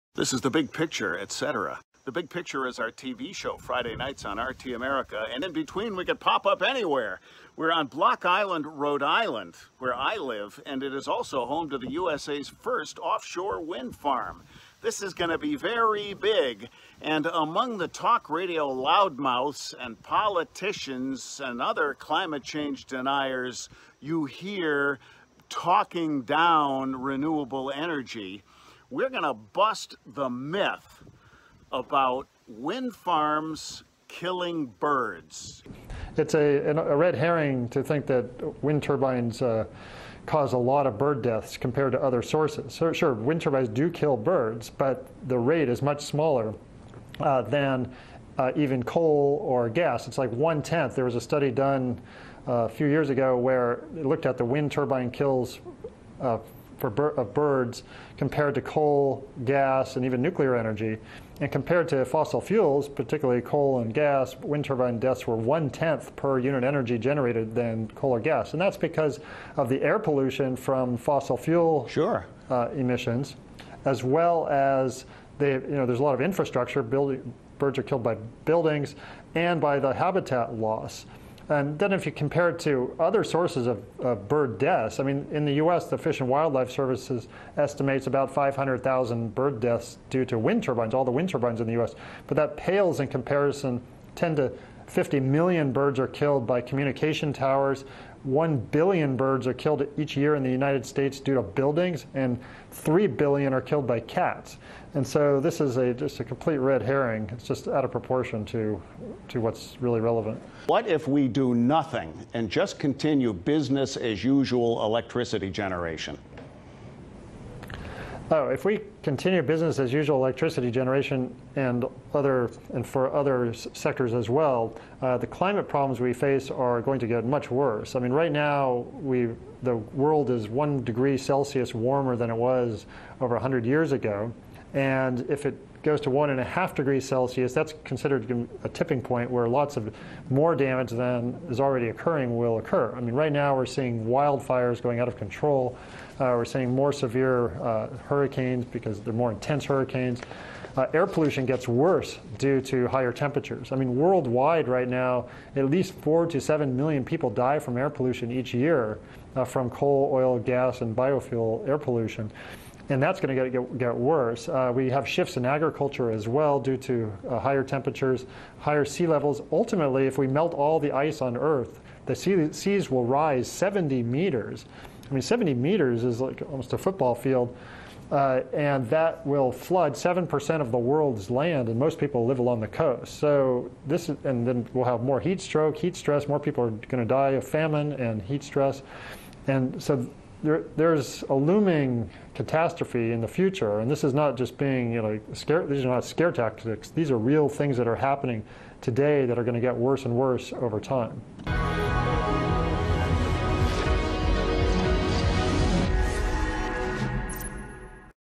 MYTH BUSTED: Windmills kill lots of birds.
After extensive environmental impact calculations, the USA's first offshore wind farm is up-and-running, 3 miles off Block Island RI. The second will be in Lake Erie, off Cleveland. Although -- or perhaps BECAUSE -- The Great Lakes have enough wind to power the entire country, the USA's biggest coal company is spending big to delay the project. Stanford University professor Mark Jacobson debunks one objection: Windmills kill birds. Various Talk Radio loudmouths and politicians and other Climate Change deniers have gone as far as to squawk that "they'll kill American Eagles." And he paints a grim picture of what will happen if business-as-usual continues.